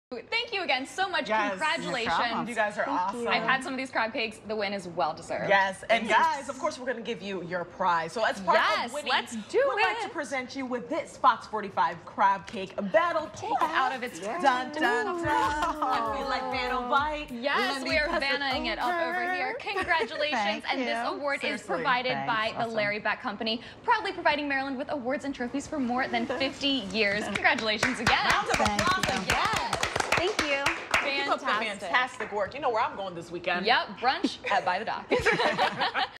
Baltimore's Best Crab Cake Award by Larry Beck
The Baltimore's Best Crab Cake award is provided by Larry Beck company.